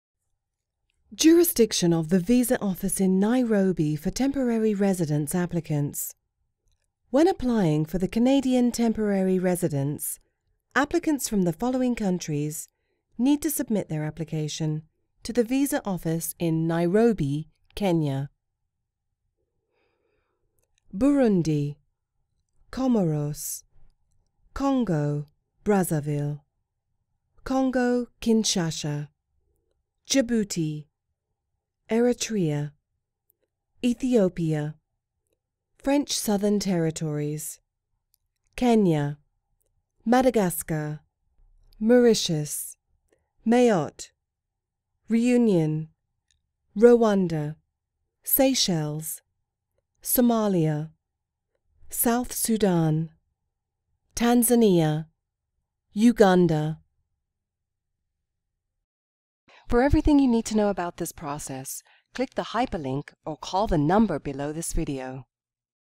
Jurisdiction of the visa office in Nairobi for temporary residence applicants
Q: Jurisdiction of the visa office in Nairobi for temporary residence applicants

A: When applying for the Canadian temporary residence, applicants from the following countries need to submit their applications to the visa office in Nairobi, Kenya:
• Burundi,
• Comoros,
• Congo (Brazzaville),
• Congo (Kinshasa),
• Djibouti,
• Eritrea,
• Ethiopia,
• French Southern Territories,
• Kenya,
• Madagascar,
• Mauritius,
• Mayotte,
• Reunion,
• Rwanda,
• Seychelles,
• Somalia,
• South Sudan,
• Tanzania,
• Uganda.